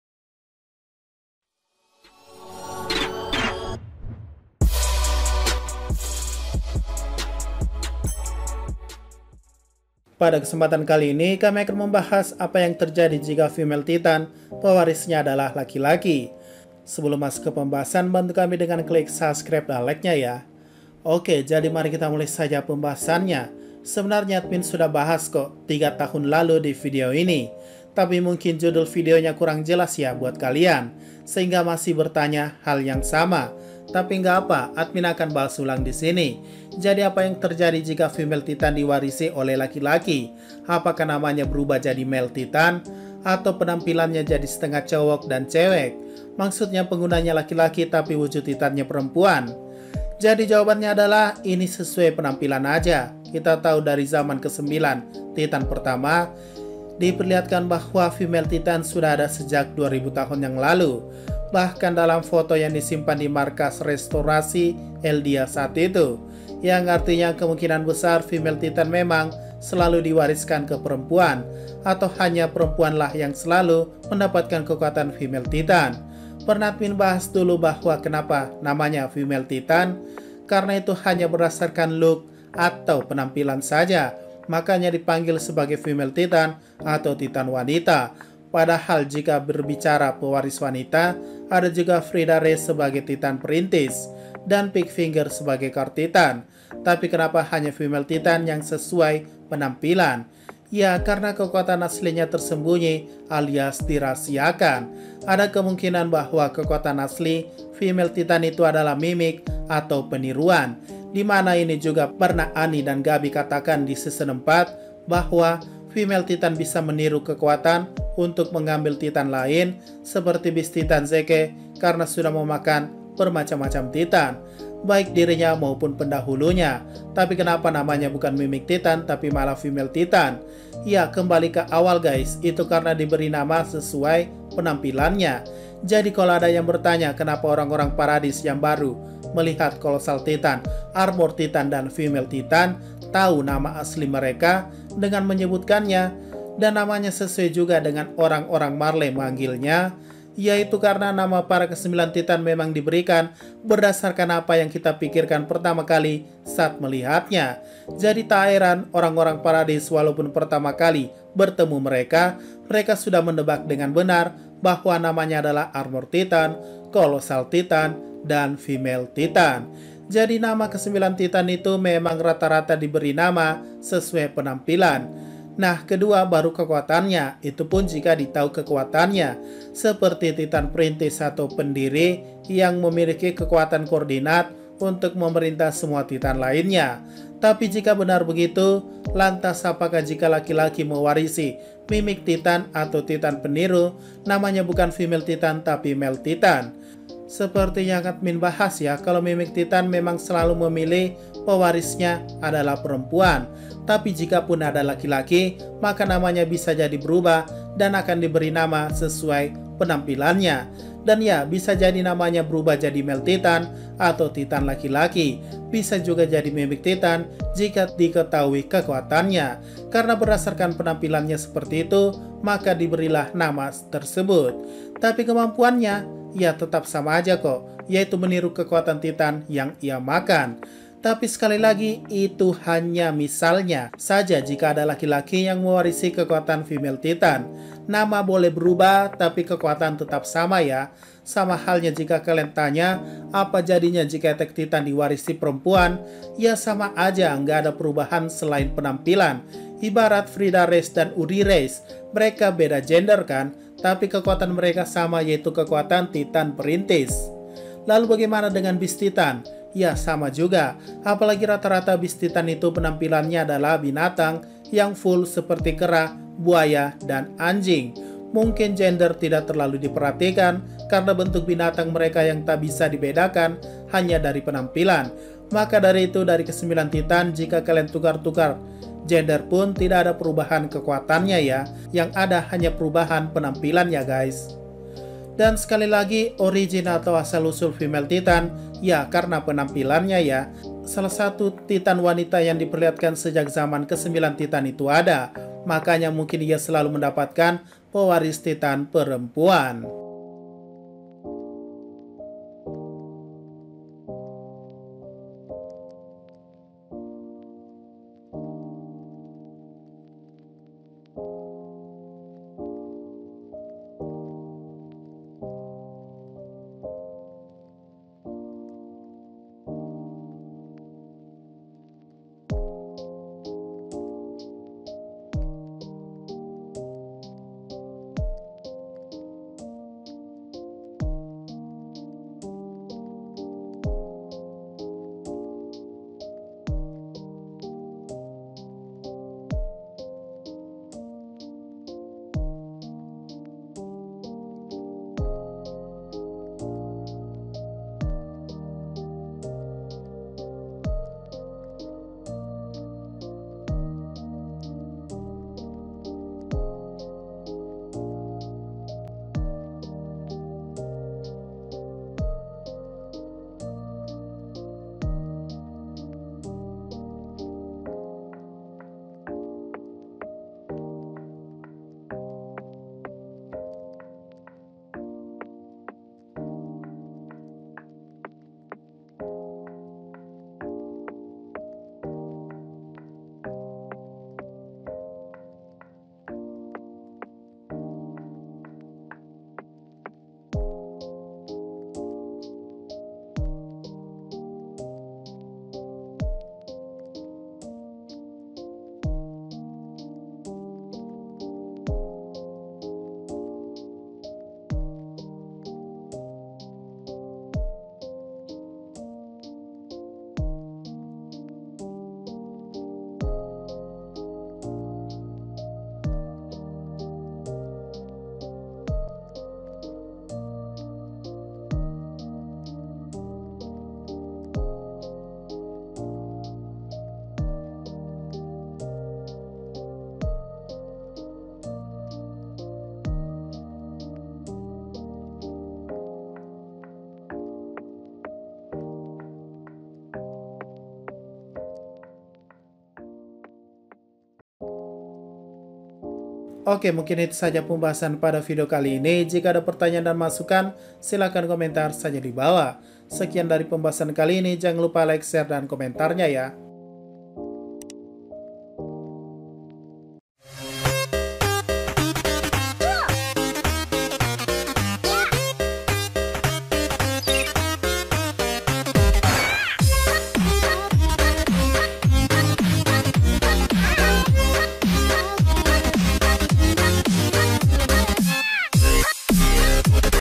APA YANG TERJADI JIKA FEMALE TITAN PEWARISNYA ADALAH LAKI-LAKI..?? Ini Penjelasannya..!!
Femali titan jika jadi pewarisnya laki. ??Simak Videonya!